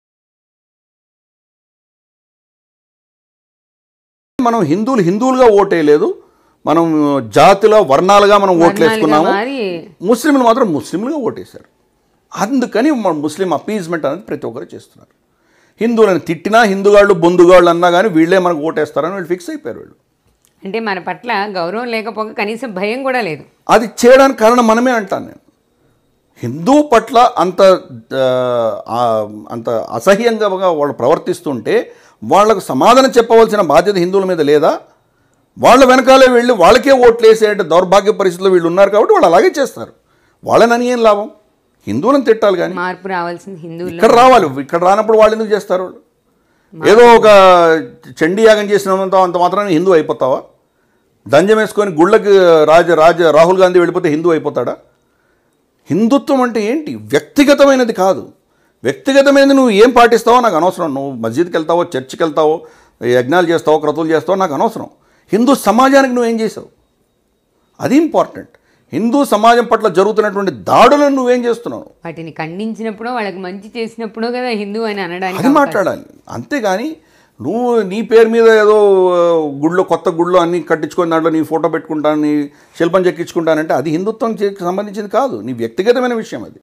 చండి యాగం చేస్తే కెసిఅర్ హిందువు అయిపోతాడా ...జంజం వేసుకుంటే రాహుల్ గాంధీ హిందువు అయిపోతాడా..
చండి యాగం చేస్తే కెసిఅర్ హిందువు అయిపోతాడా ...జంజం వేసుకుంటే రాహుల్ గాంధీ హిందువు అయిపోతాడా..Analyst mamidi giridhar exclusive interview